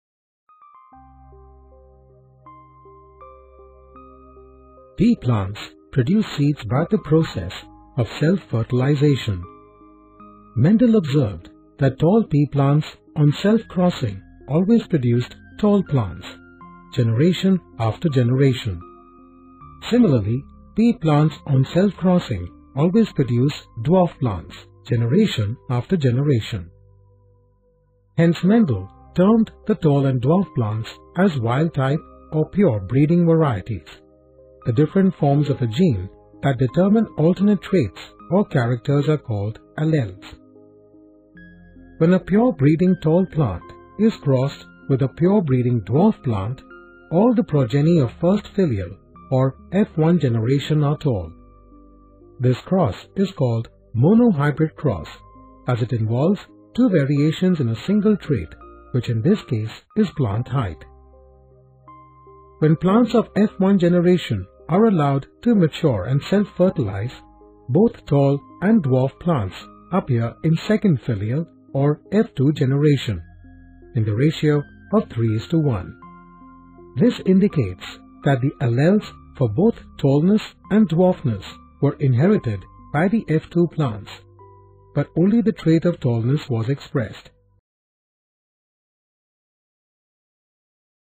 Did You Know? | Genotype & Phenotype | Biology
Understand different traits of the pure breeding varieties of plants.